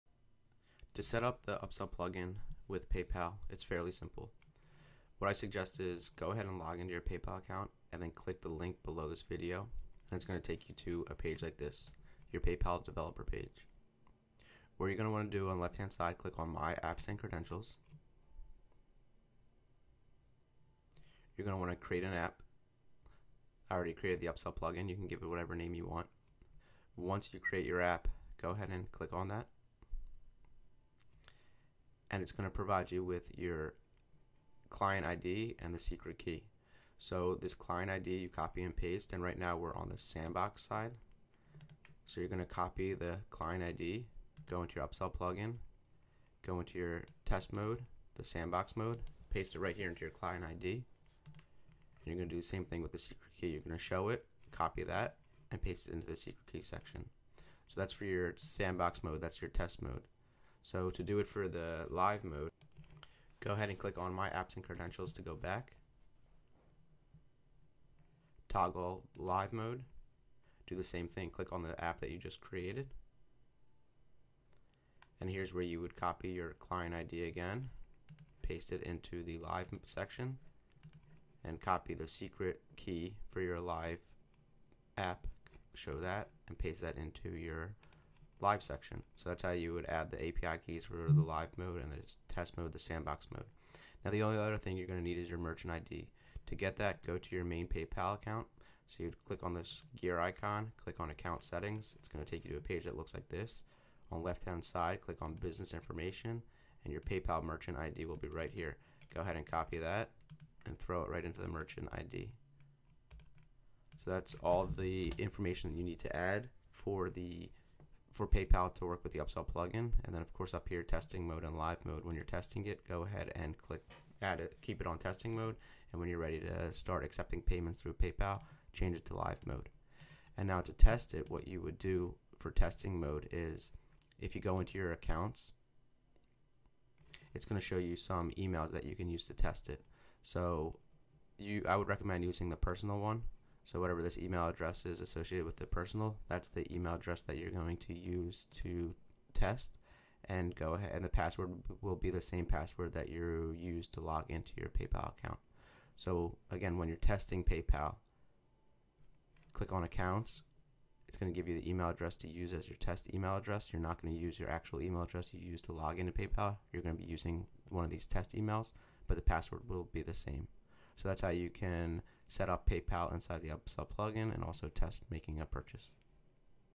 Integrating PayPal with the Upsell Plugin
Watch this video to learn how to integrate PayPal with the Upsell Plugin.

Here is the link I mention in the video you will want to click on after you log into payal to locate your API keys